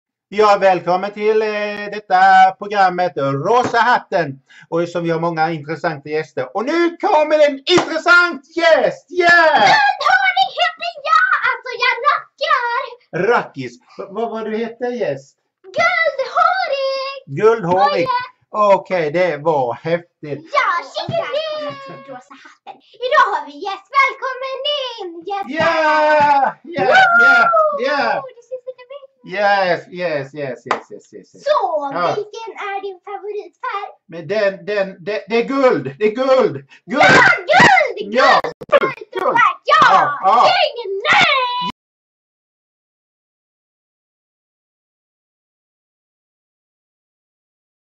Rosa Hatten
Pappa Christer Åberg och hans dotter Desiré "Dessan" Åberg gör "tv-programmet" Rosa Hatten